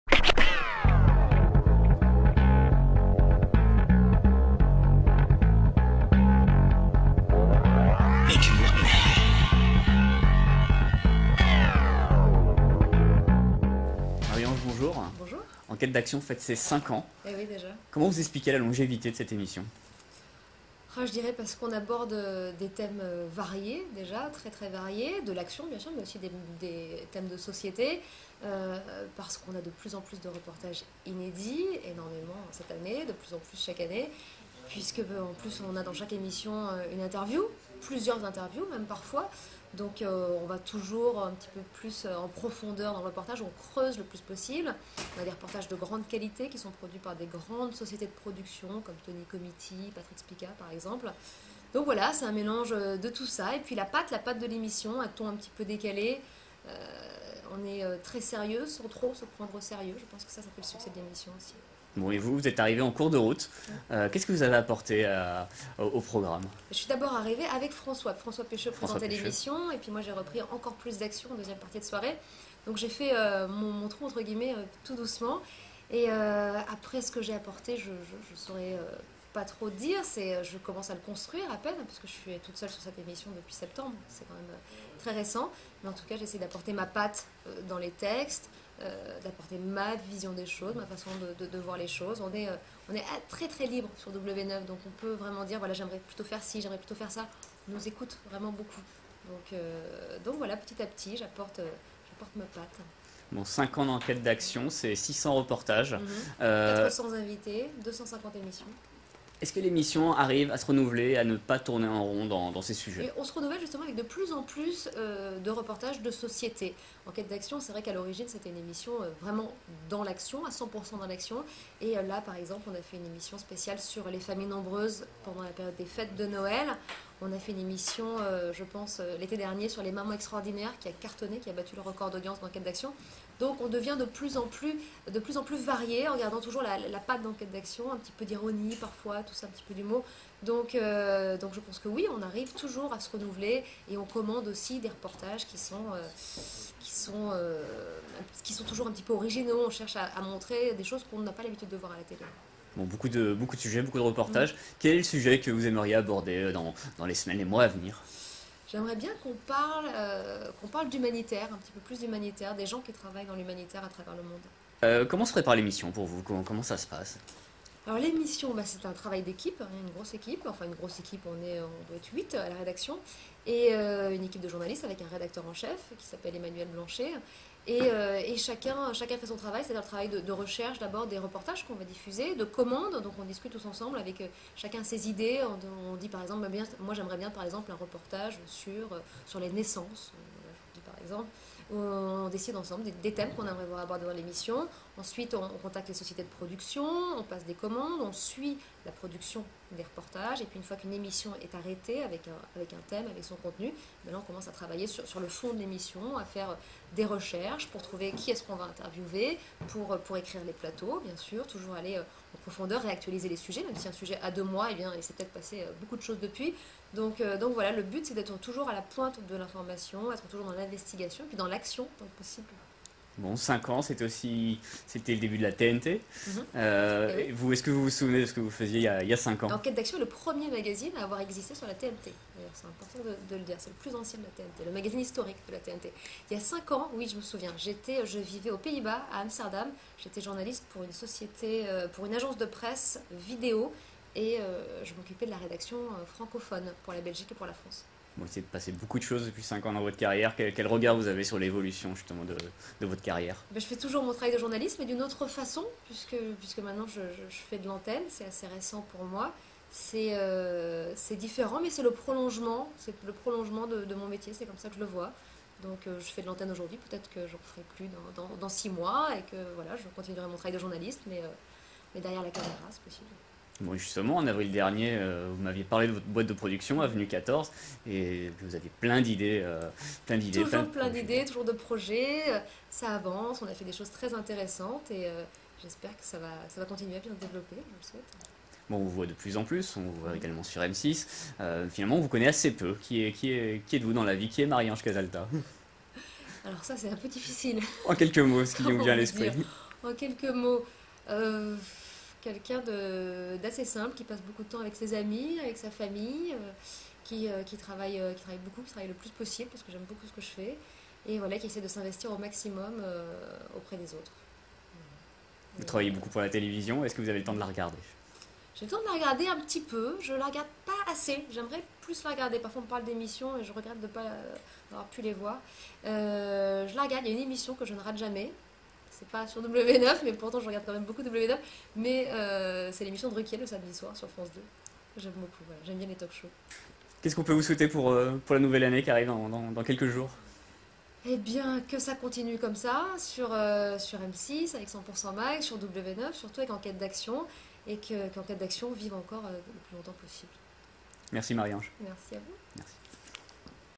Marie-Ange Casalta fête les 5 ans d'Enquête d'action sur W9
Vidéo TVMag.com ! Dans les coulisses d'Enquête d'action (W9) avec Marie-Ange Casalta